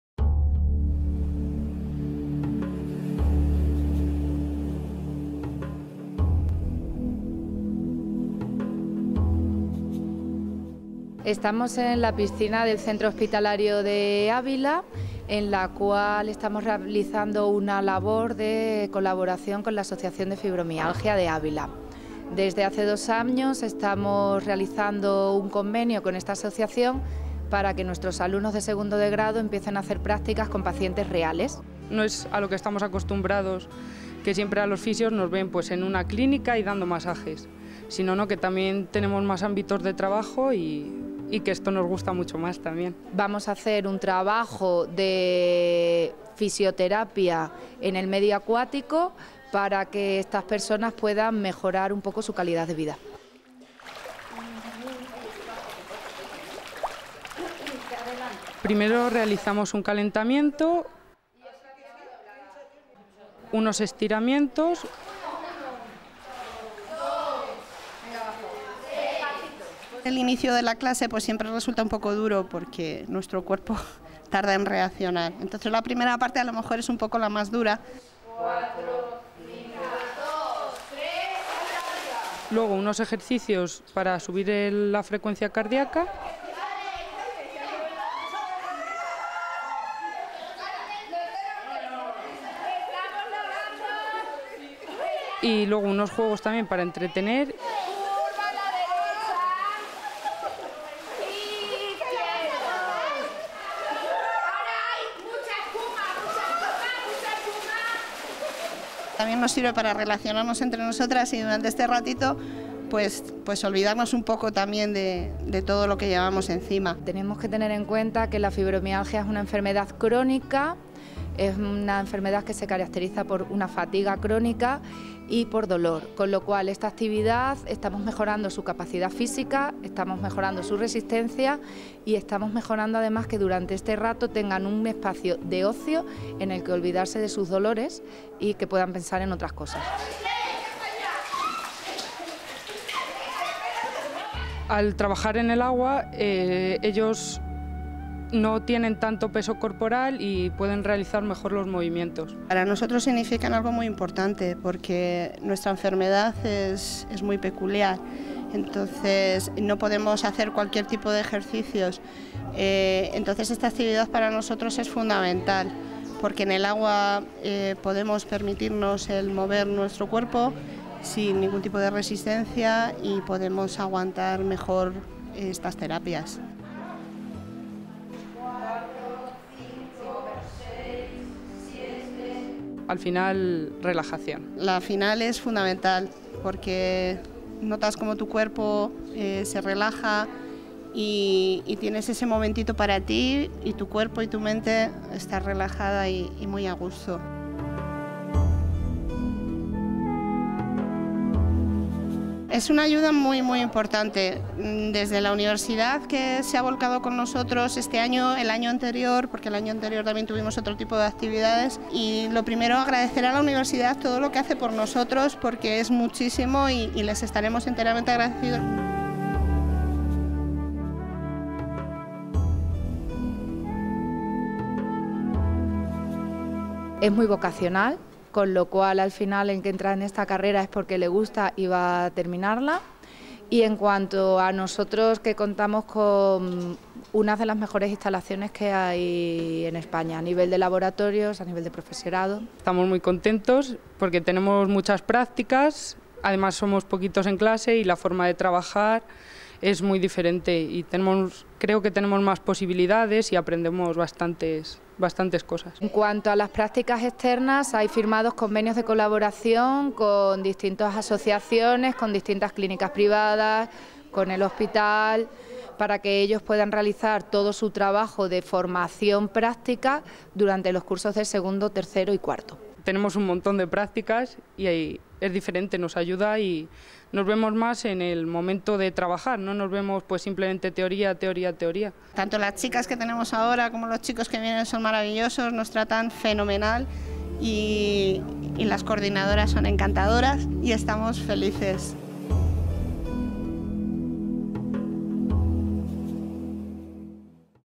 Prácticas Fisioterapia UCAVILA: terapia bajo el agua
Los alumnos de Fisioterapia de UCAVILA realizan prácticas de terapia bajo el agua, de la mano del Convenio firmado con la Asociación de Fibromialgia y Fatiga Crónica de Ávila.